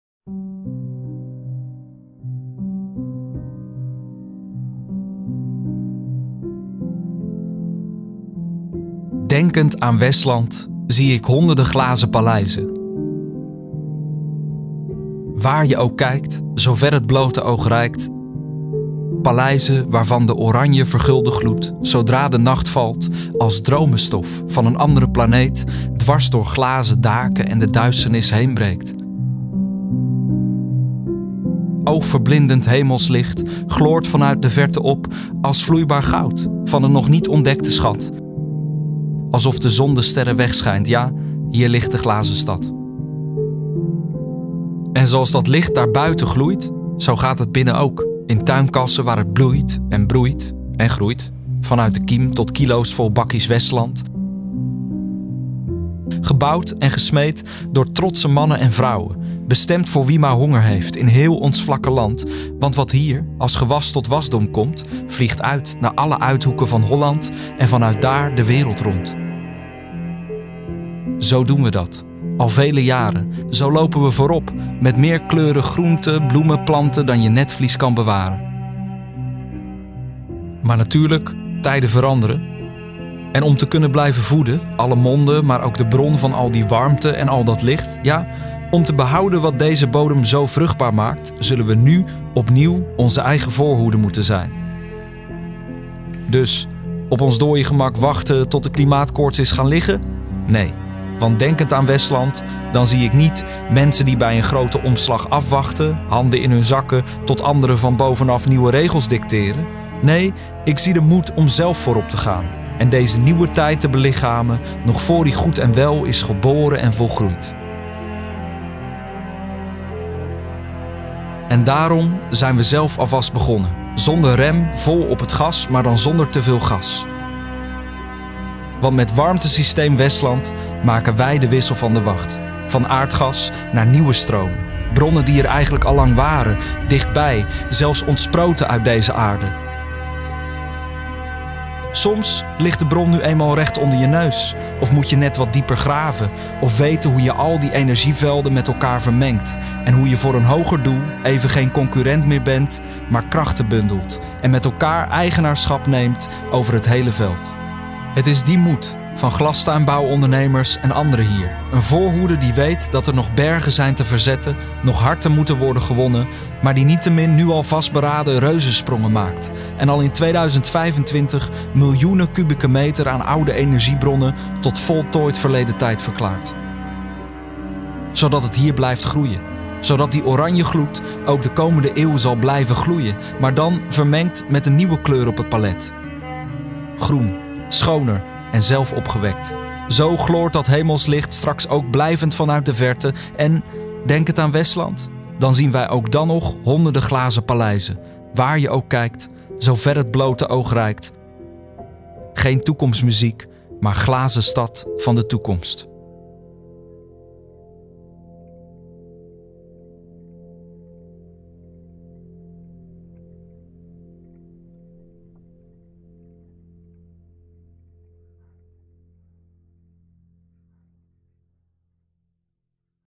Denkend aan Westland
Denkend aan het Westland zie ik honderden glazen paleizen, waar je ook kijk, zover het blote oog reikt.